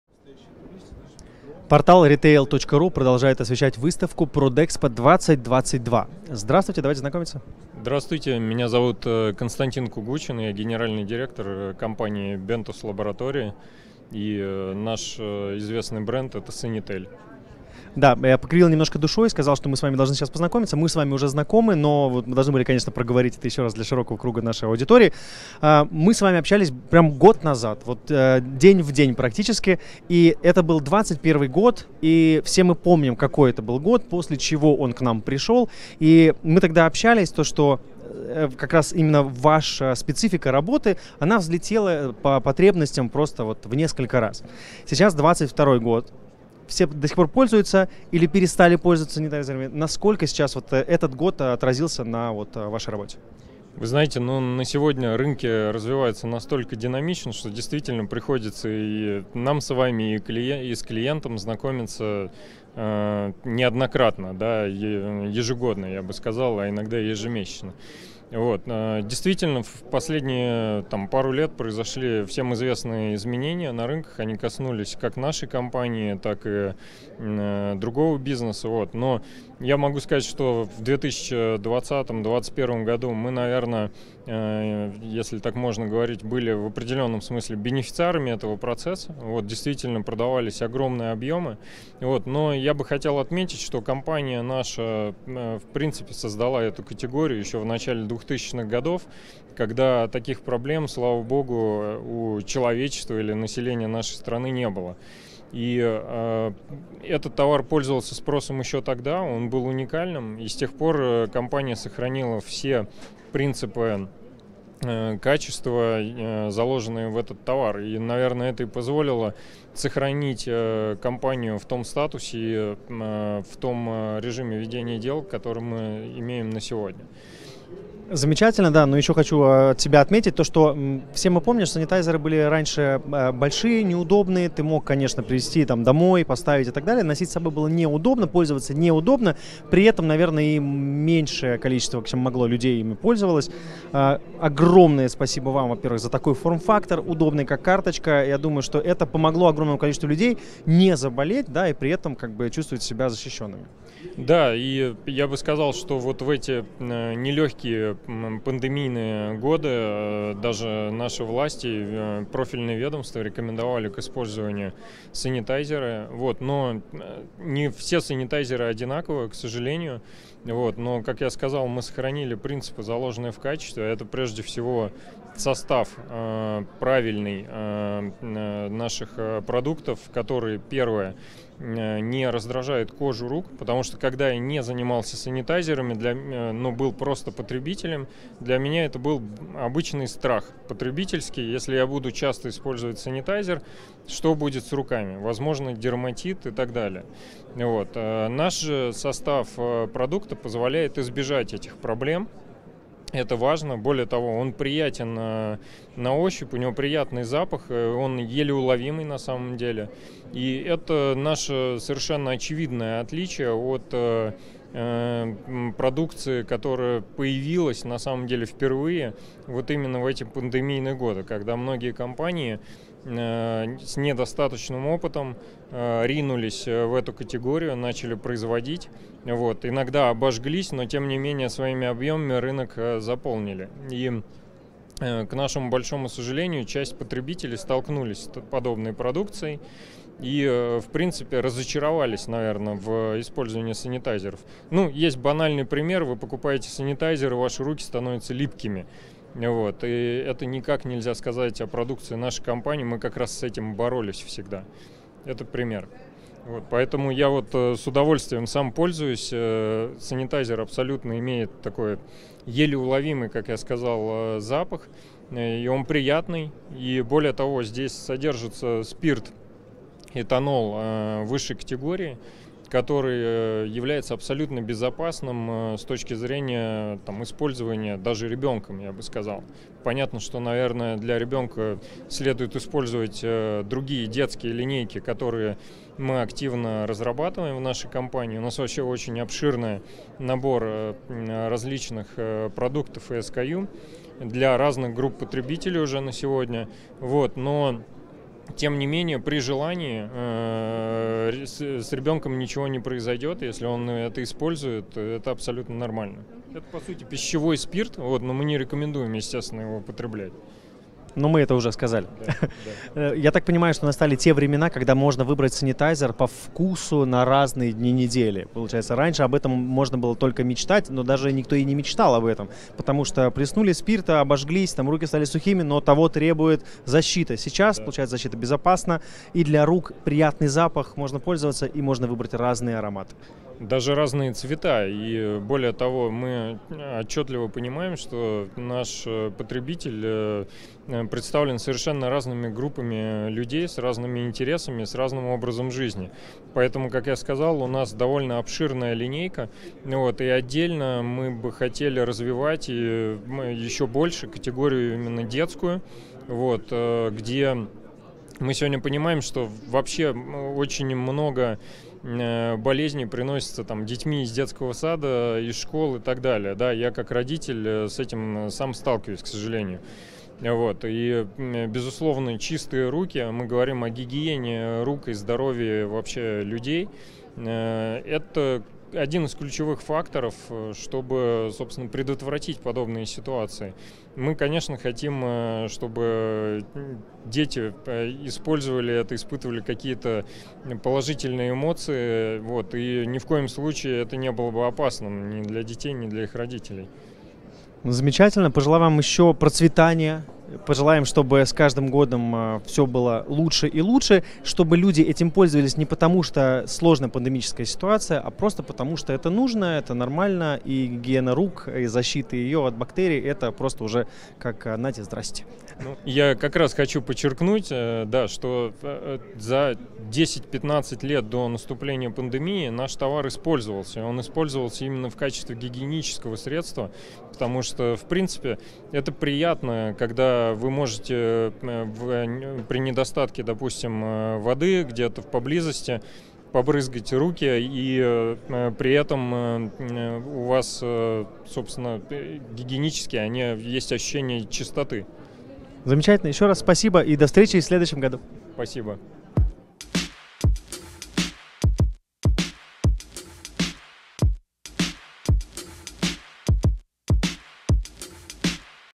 Константин Кугучин - Бентус лаборатории на
Константин Кугучин, генеральный директор "Бентус лаборатории", о том, как пандемия изменила качество и состав санитайзеров, а также их упаковке